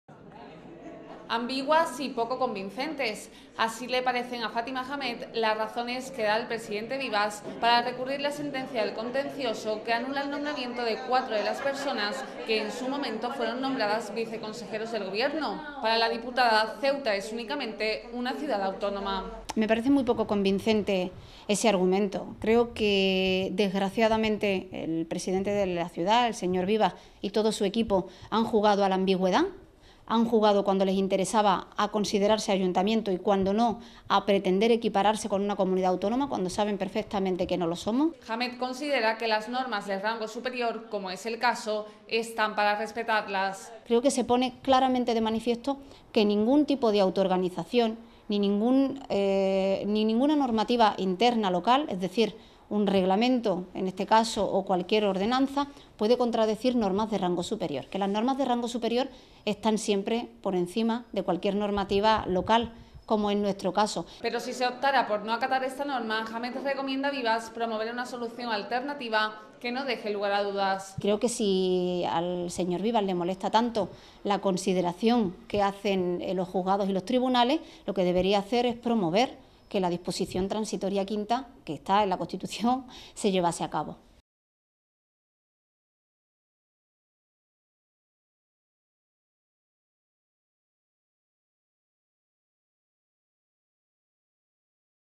Hamed, sobre los viceconsejeros: “Las normas hay que cumplirlas”
A Fatima Hamed no le convencen las explicaciones del Gobierno para recurrir la sentencia que anula el nombramiento de los viceconsejeros. Para la diputada es necesario el cumplimiento de las normas de rango superior, muy por encima de cualquier ordenanza local y que, por tanto, la sentencia del Tribunal Constencioso debe acatarse.